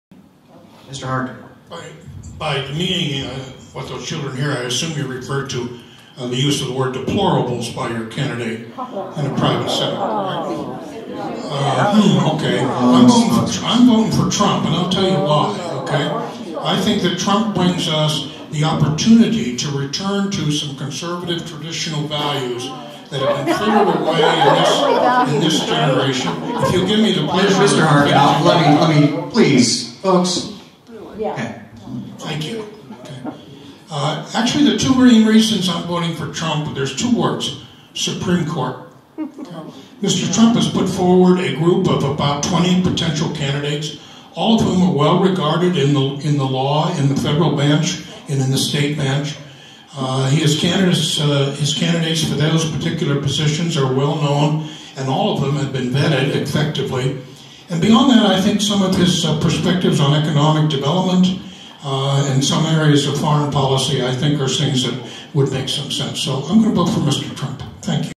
Catherine Talkington /Hartgen Response
Steve Hartgen tells why he was voting for Trump.